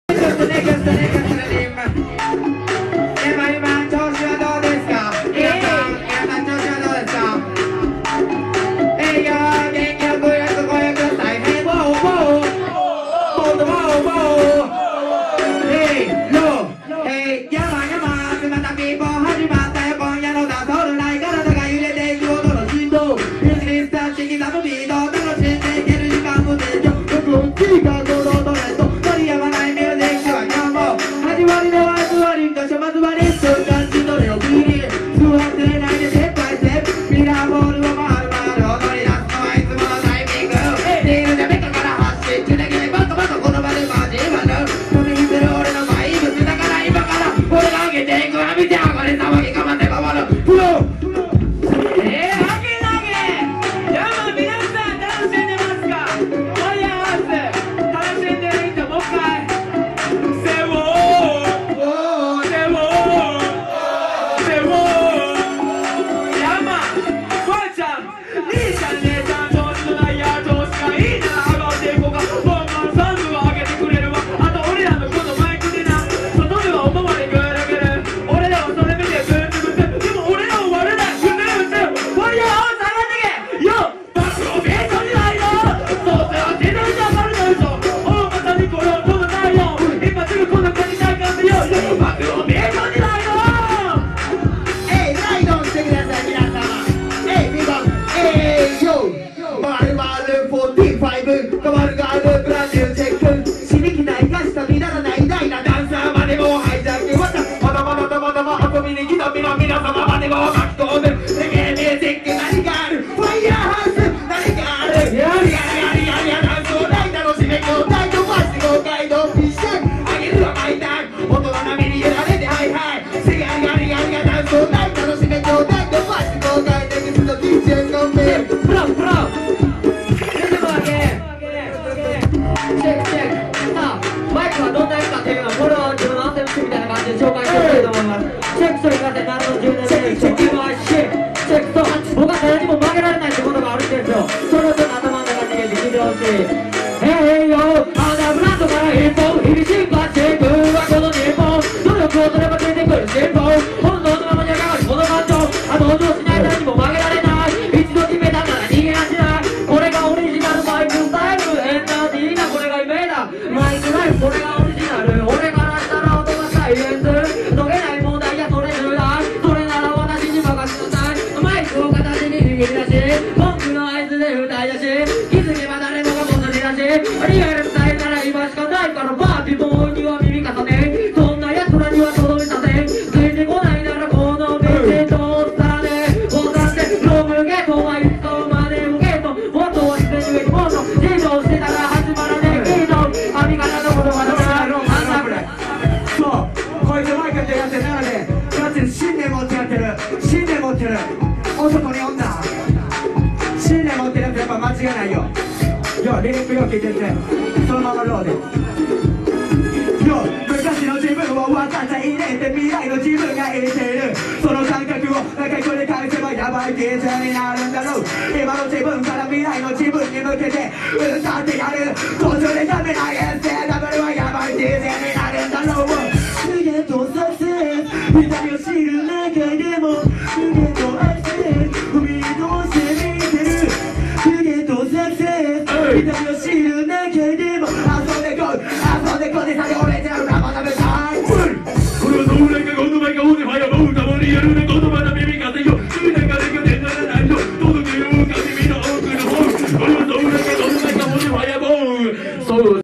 FIRE HOUSE #15 -HAPPY VALENTINE'S PARTY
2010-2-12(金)
FIRE STORM×CLUB DROP PRESENTS
@CLUB DROP
START: 23:30

■HOST SOUND     FIRE STORM

■SOUND     BEAT CROWN / DJ JUNPAY / KINGDOM SOUND / TRICK STAR and SOUND CREW!!

■DJ & SINGER     MONKEY.D / 168 / KUNIMITSU / J-KING / 音人 / JAI / MIC / KOHEY / TARA-坊 and more DJ & SINGERZ!!